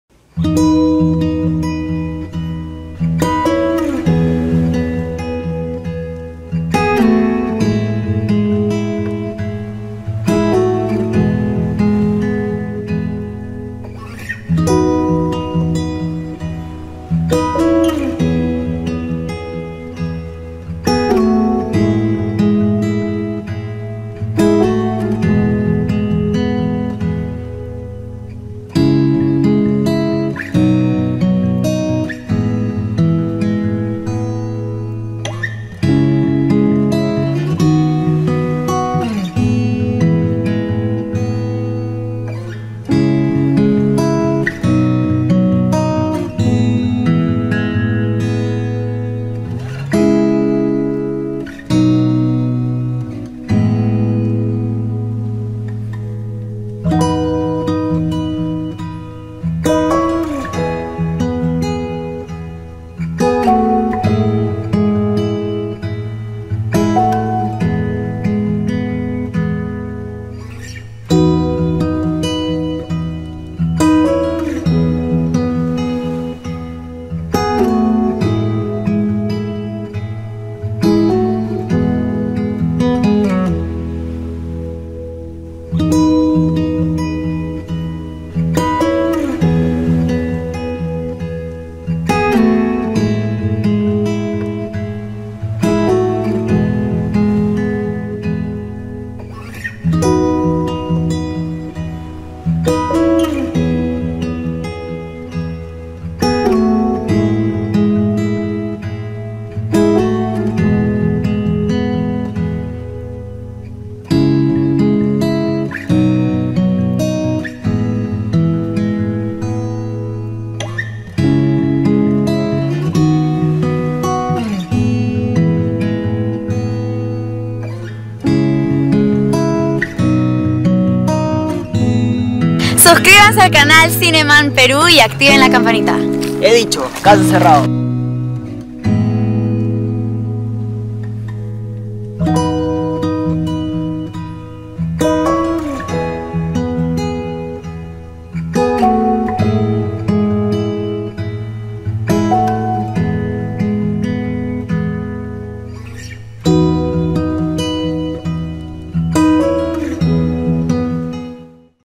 James Braddock Park - North Bergen NJ 😻 DRONE cinemanperu
80th St PARK - North Bergen NJ. 😻 Between Bergenline Ave, Boulevard East, 80th St and 90thSt. 🤖 Drone recording.